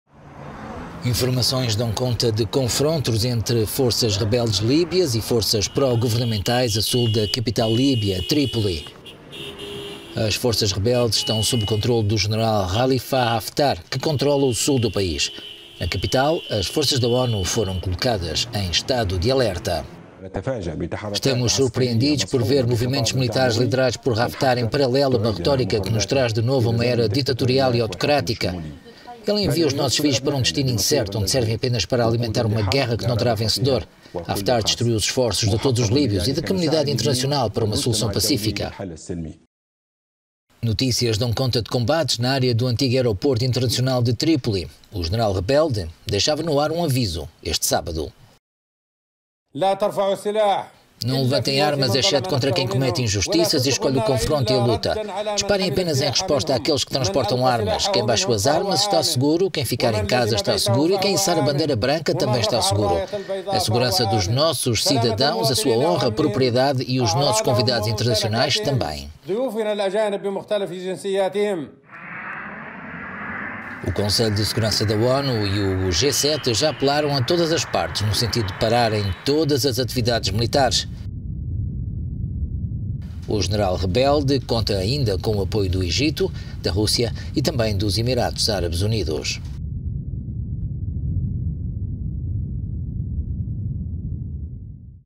Forças rebeldes avançam sobre Tripoli
O Conselho de Segurança da ONU e o G7 apelam à interrupção imediata dos combates…